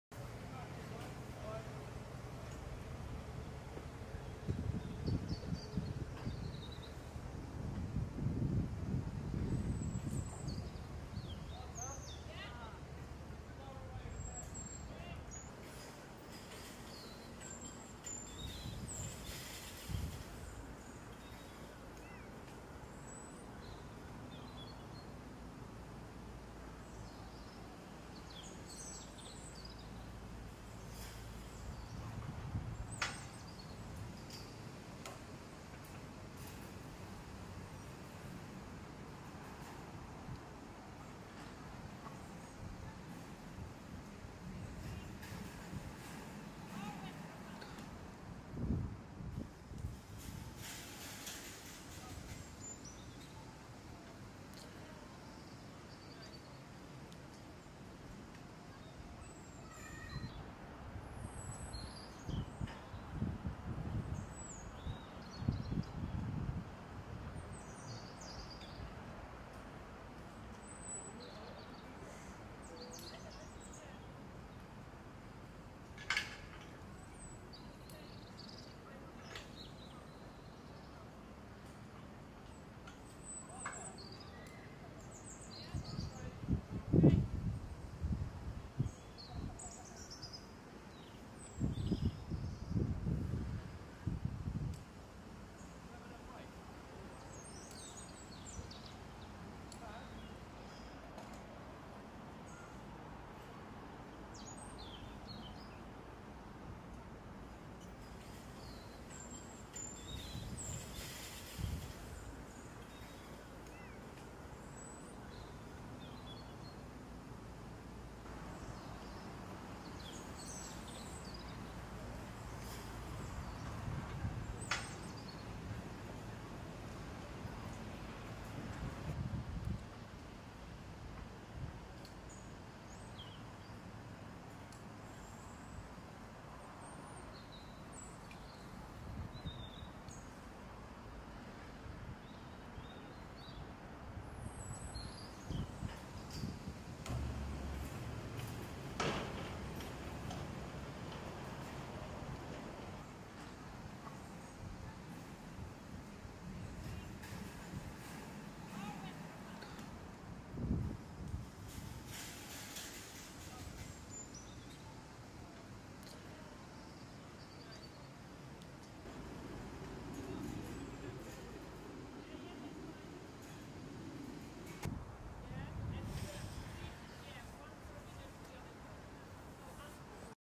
Work begins to turn historic SNOWDON AVIARY at London Zoo into new Monkey Valley enclosure
Abseilers have been working on the famous Snowdon Aviary at London Zo in Regent's Park as the structure is converted into a new Monkey Valley enclosure. A troop of Eastern black and white colobus monkeys.  The striking bird aviary has been in place by the canal for 50 years.
Footage supplied by the ZSL London Zoo